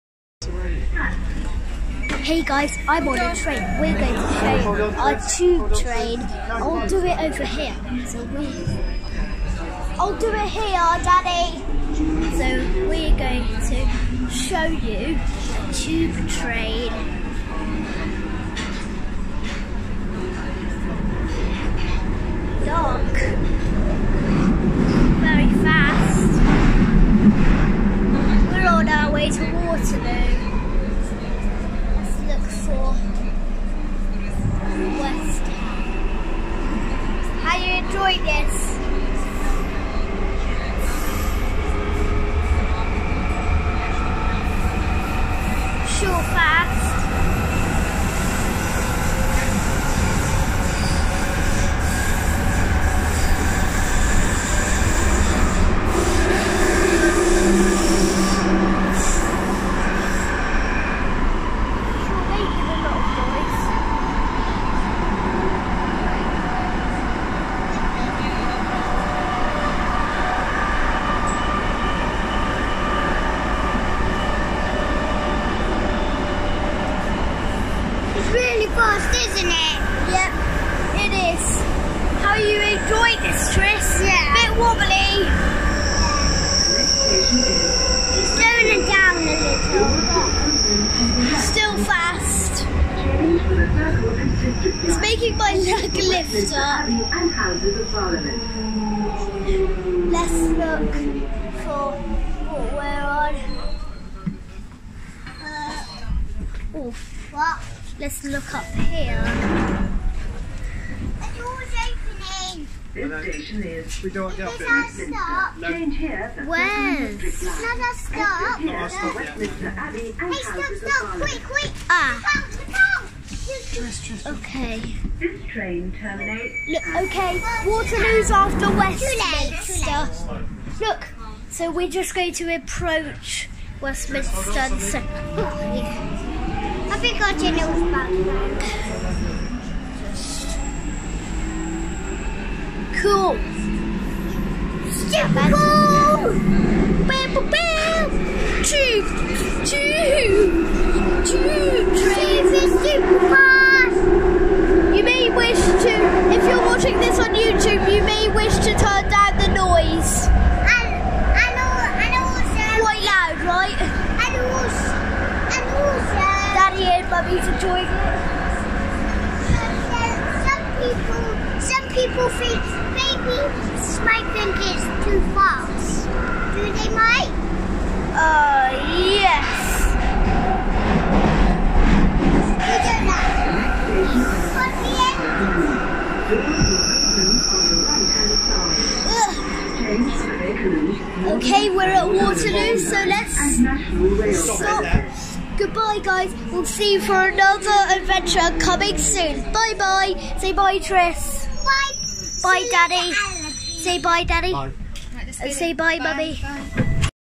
[CRINGE] Journey on the Jubilee Line - Green Park to Waterloo (Filmed 27th May 2019)
Prepare for cringe.
Filmed: 27th May 2019

Chapters
0:00 Intro
0:02 Departing Green Park
1:26 Arriving at Westminster
1:48 Arrived at Westminster
2:05 Departing Westminster
3:00 My Weird Bendy Thumb
3:03 Arriving at Waterloo